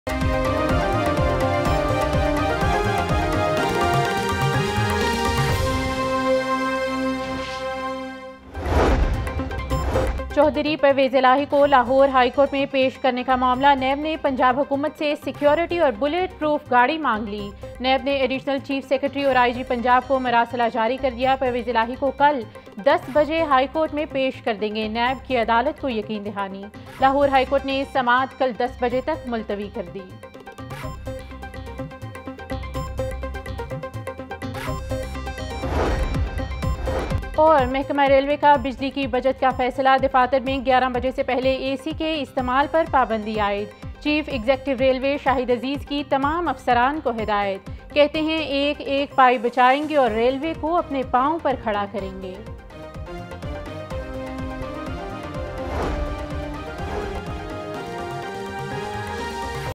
Increase In Dengue Cases! | 07:00 PM News Headlines | 31 August 2023 | Lahore News HD
People Are Tired Of Over Billing

Protest Against Over Billing

Intermittent Rain In Different Parts Of The City Turns Weather Pleasant

Monsoon Third Spell Expected

Broiler Rates Decrease | Broiler Rates Updates

PMLQ in Action